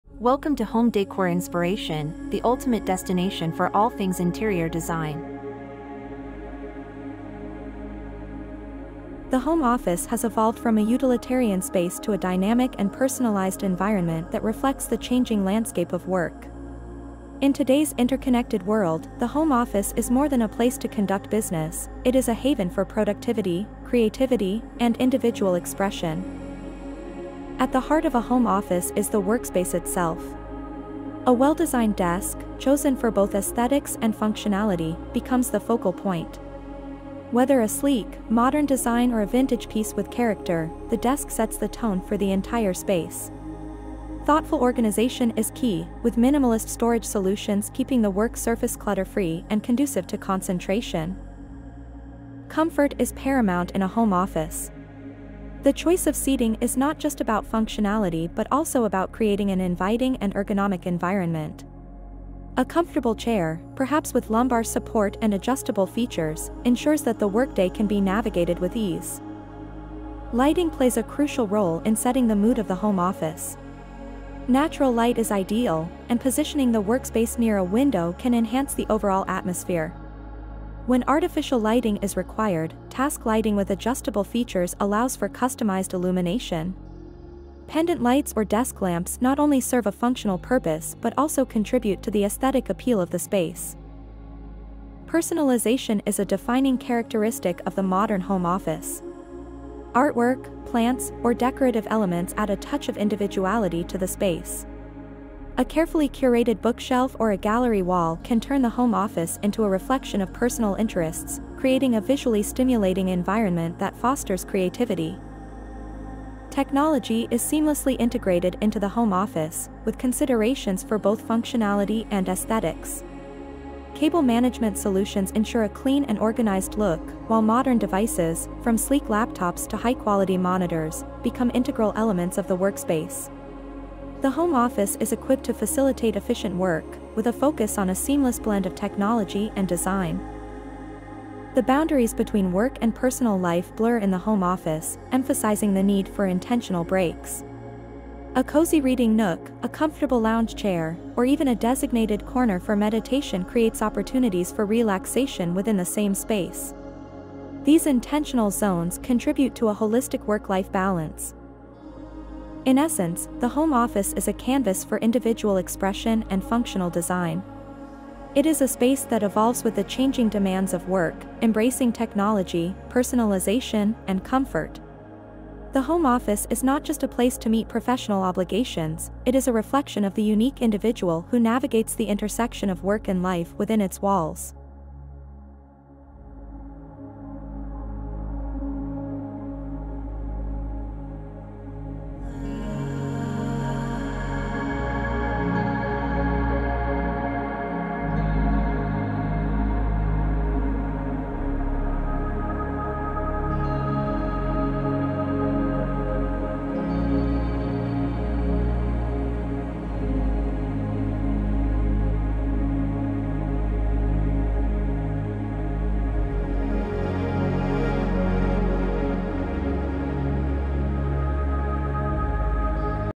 30+ Best Home Office Design Ideas | Aesthetic Home Office Ideas for a Cozy Interior
Transform your workspace into a haven of creativity and comfort with our curated collection of aesthetic home office ideas. Dive into the perfect blend of functionality and style, as we explore cozy interior designs, chic decor elements, and clever organizational hacks. From dreamy desk setups to soothing color palettes, discover how to make your home office a place where productivity meets aesthetics seamlessly.